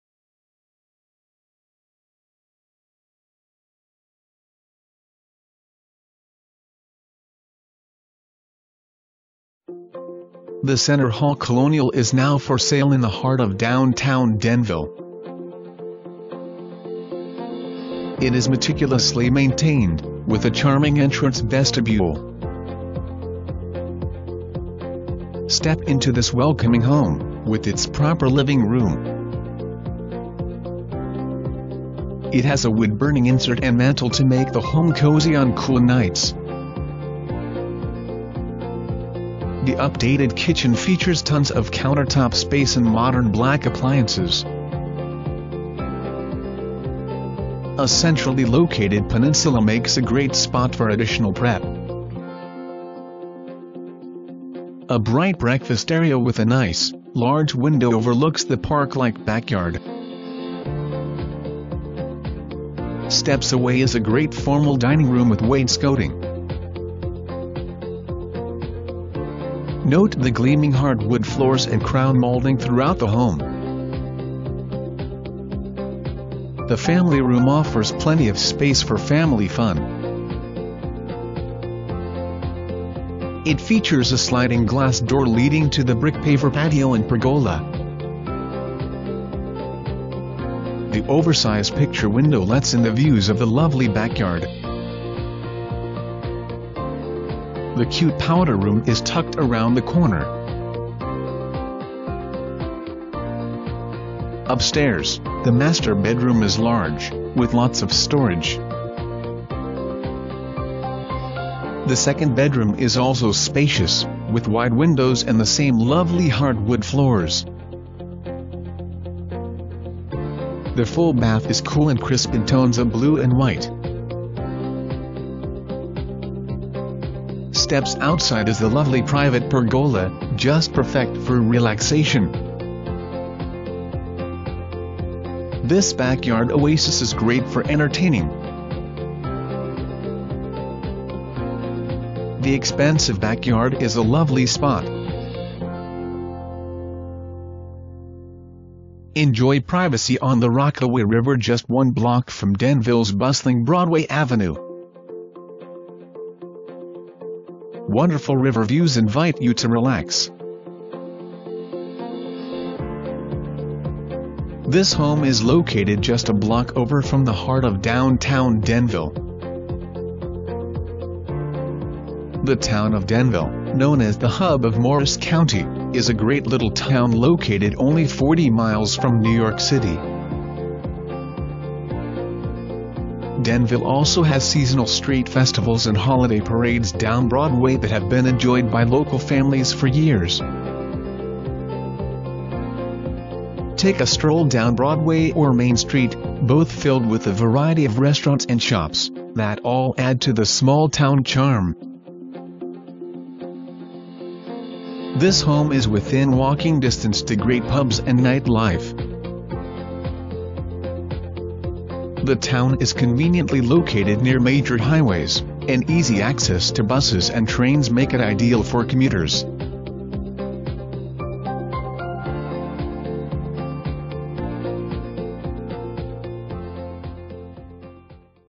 Home for Sale: 8 Third Avenue in Denville
Meticulously Maintained. Charming Center Hall Colonial in the Heart of Downtown Denville. Entrance Vestibule. Proper LR w/Wood Burning Insert &Mantel. Great Formal DR w/Wainscoting. Gleaming HW Floors &Crown Molding Throughout. Updated Kitchen Features Tons of Countertop Space, a Centrally Located Peninsula &Modern Black Appliances. Bright Bfast Area w/Large Window Overlooks Park-Like Setting. Family Room Offers an Oversized Picture Window &Sliding Glass Door to Brick Paver Patio & Pergola. Stunning Backyard Oasis Perfect for Entertaining & Relaxation! Enjoy the Privacy on the Rockaway River Just 1Block from Denville?s Bustling Broadway Ave. Plenty of Parking On & Off St. +1 Car Garage. Sidewalks-Walk to Downtown & NYC-Direct Bus Stops. Enjoy Denville's Shopping, Restaurants, Farmers Market &Top Rated Schools.

Presented By:
Mary K. Sheeran, Keller Williams Metropolitan